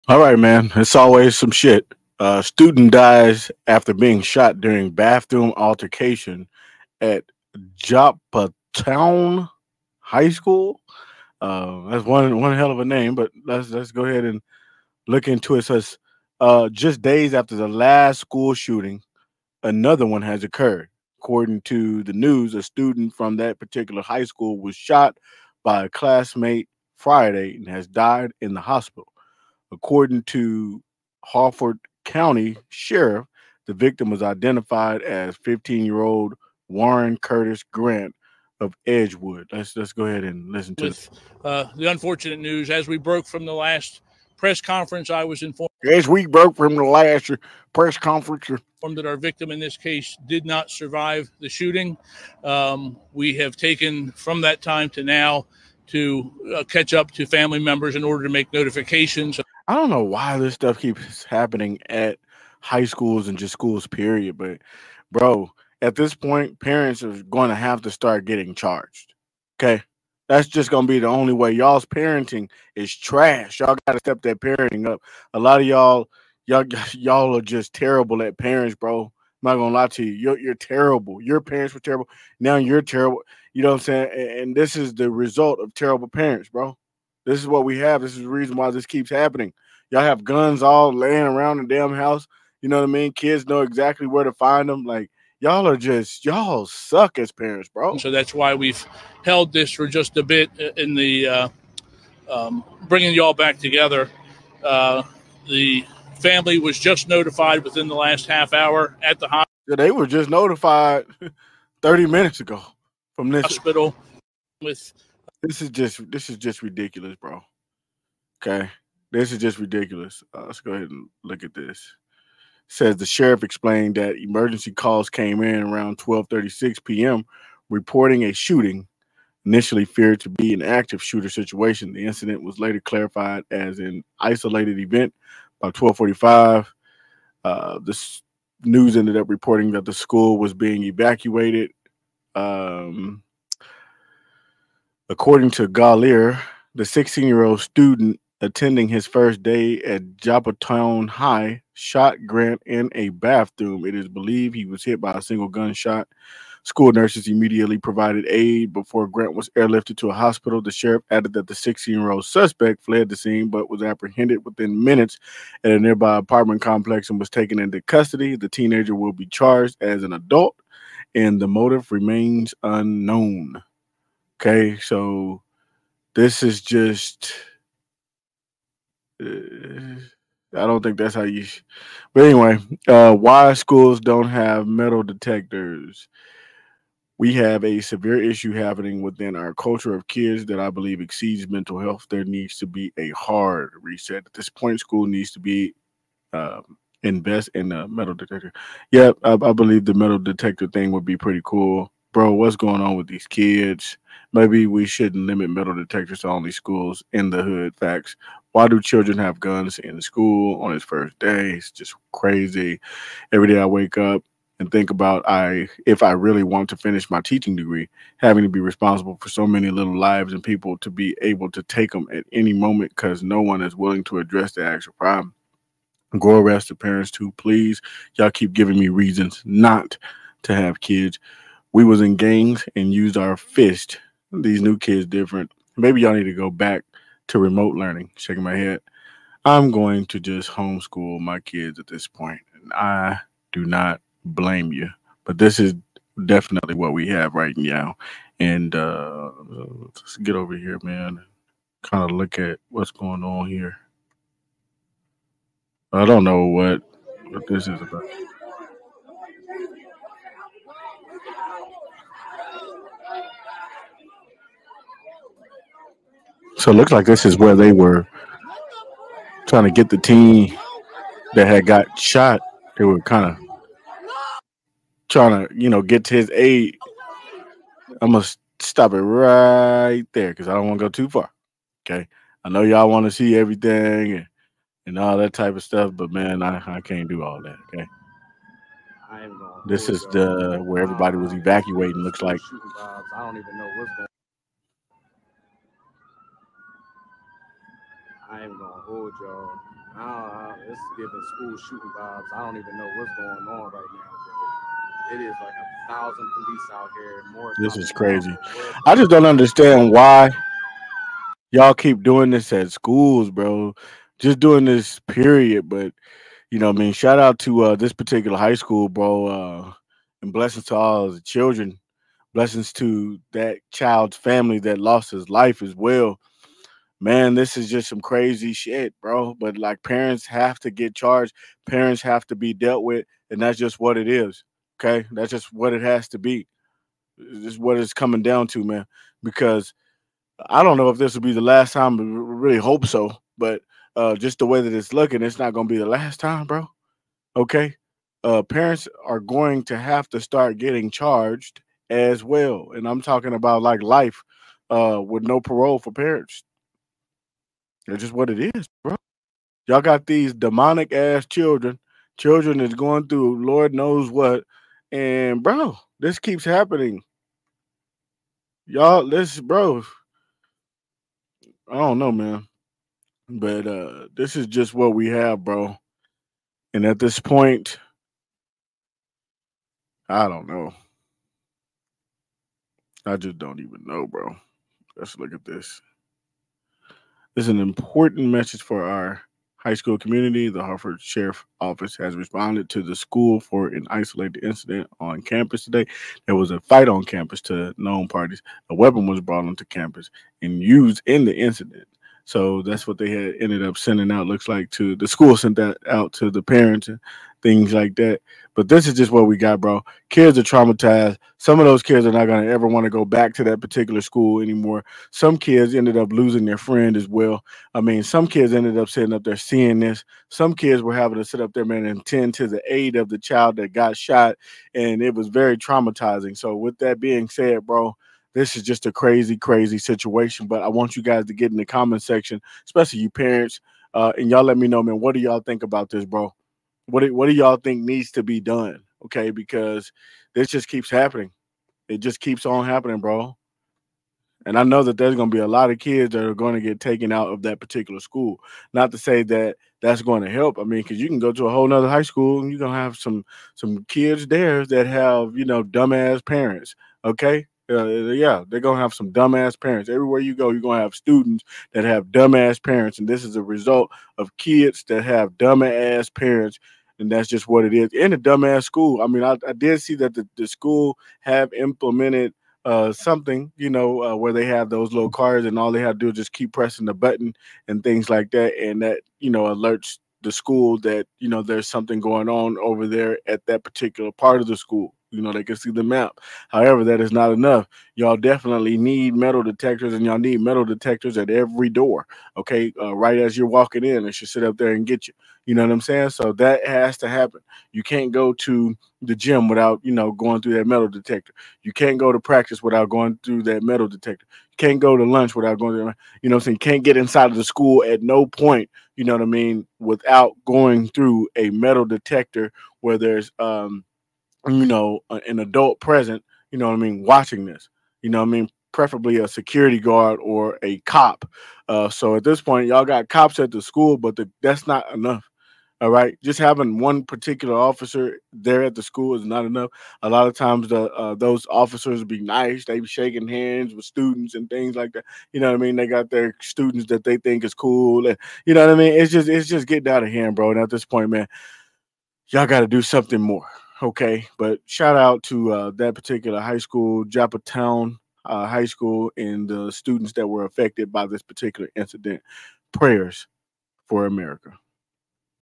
15 Year Old Gets Deleted At School During An Altercation At Joppatowne High School.
A Joppatowne High School student shot by a classmate Friday died at a hospital, Harford County Sheriff Jeff Gahler said.

The sheriff identified the student who died as Warren Curtis Grant, 15, of Edgewood